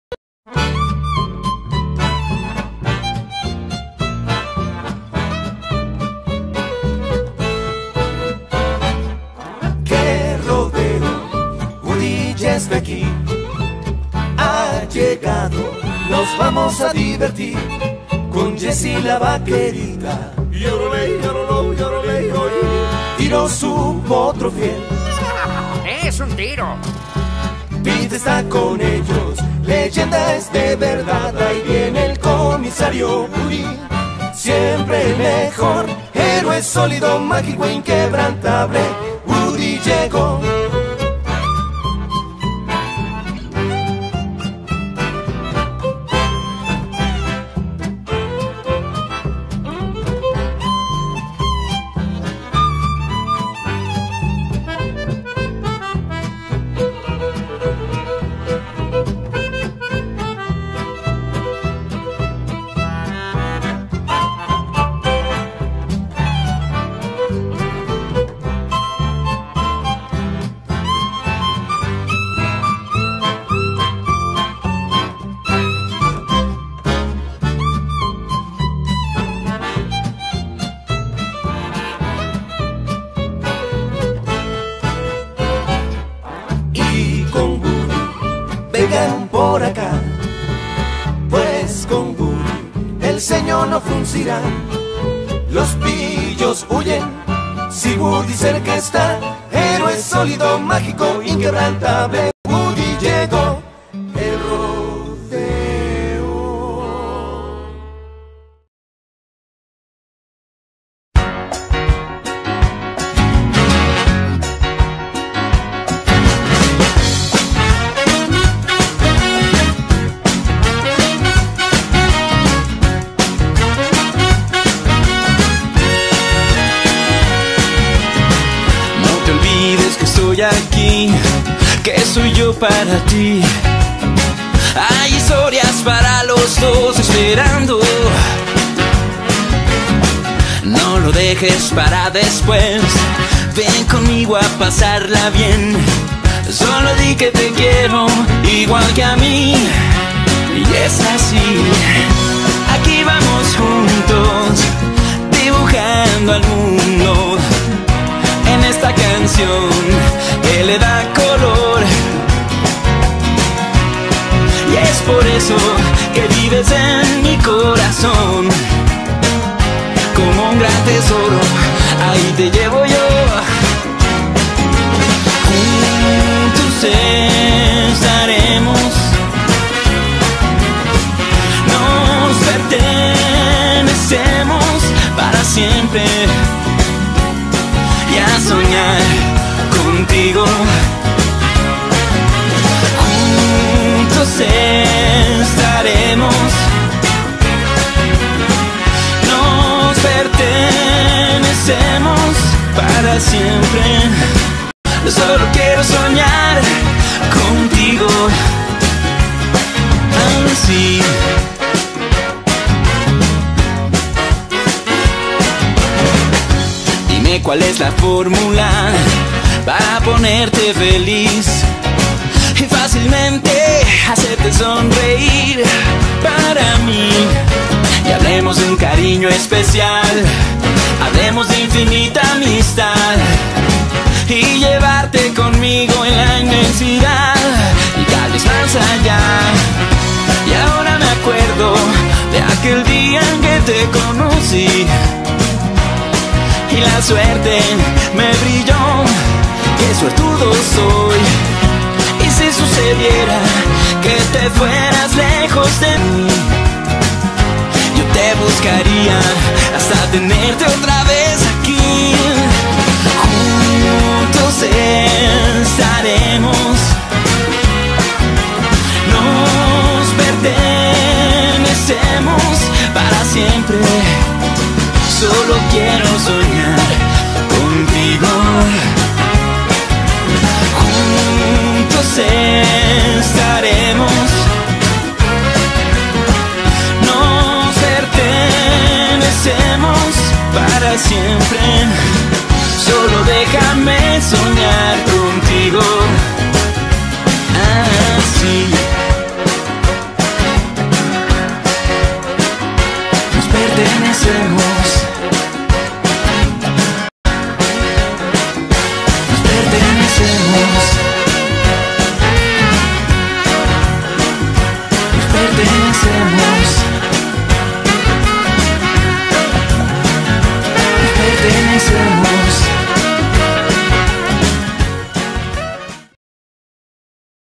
Cómo dibujar a Jessie la vaquerita | Toy Story
Hoy les traigo el proceso de uno de mis personajes favoritos de Toy Story, espero que sea de tu agrado y que me sigas apoyando en mi canal.

Realice el dibujo en papel opalina y colores Prismacolor Premier. Además, tarde aproximadamente 12 horas repartidas en 3 semanas.

y recuerda que el ARTE es la mejor CURA para todo.